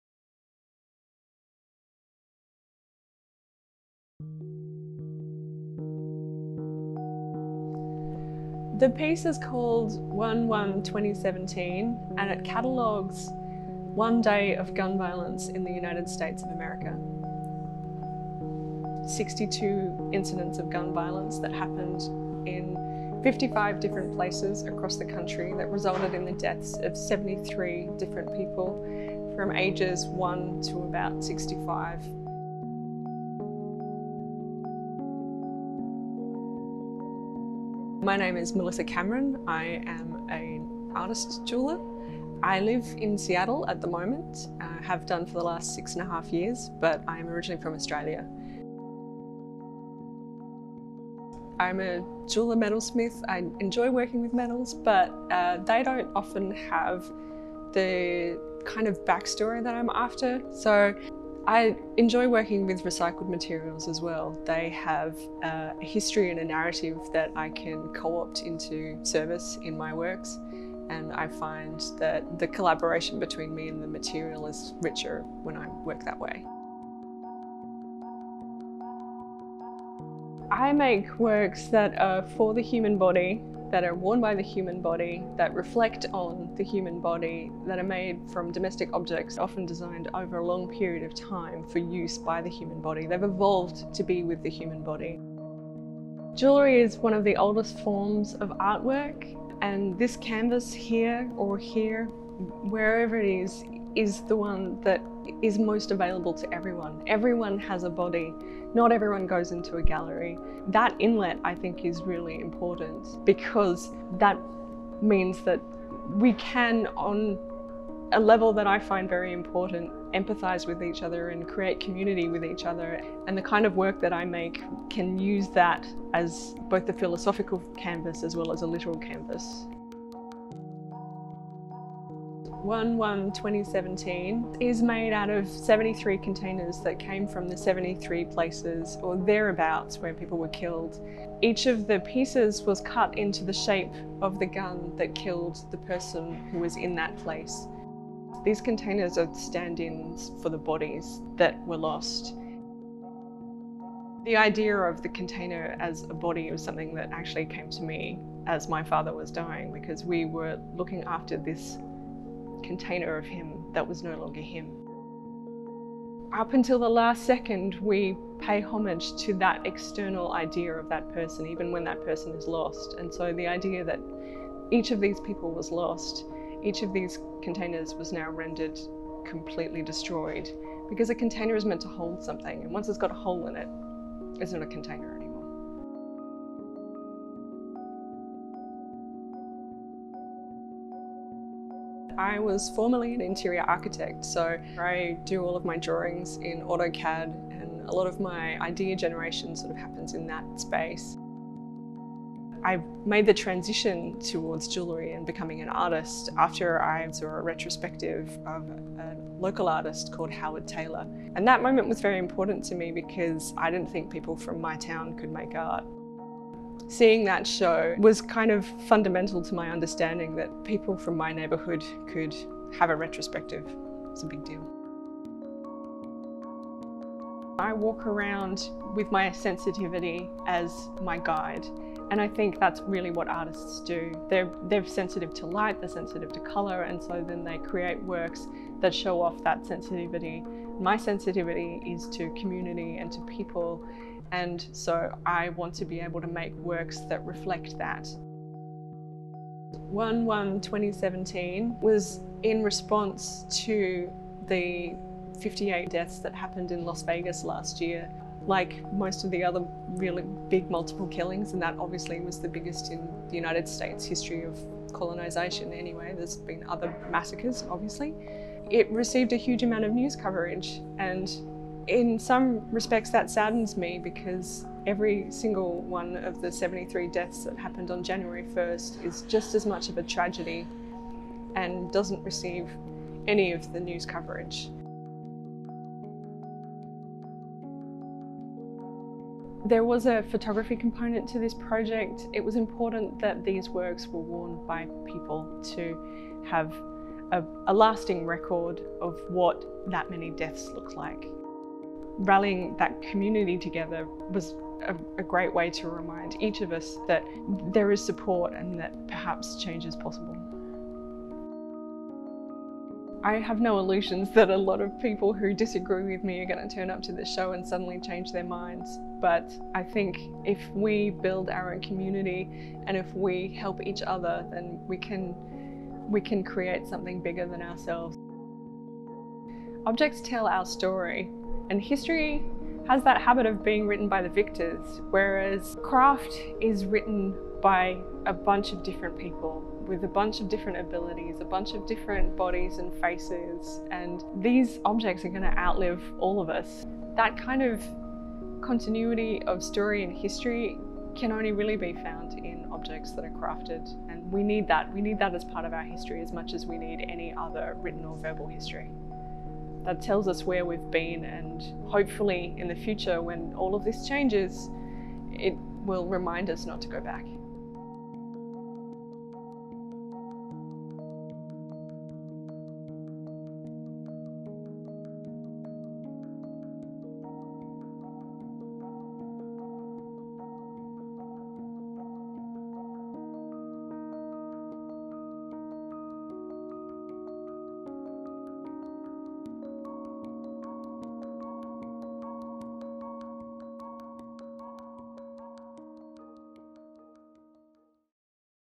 Transformation 10: Contemporary Works in Found Materials
Meet the Contemporary Craft's 2017 Raphael Prize winner - jewelry artist Melissa Cameron.

Cameron’s winning artwork "1.1.2017" catalogues one day of gun violence in the USA. A total of 62 fatal gun incidents happened in 55 places throughout the U.S on January 1, 2017 with 66 guns involved and 73 people impacted.

Cameron's "1.1.2017" and works by 25 exhibiting artists are now on view at Contemporary Craft's exhibition - Transformation 10: Contemporary Works in Found Materials - until March 23, 2019.

Video produced by Michael Pisano (Pisano Films)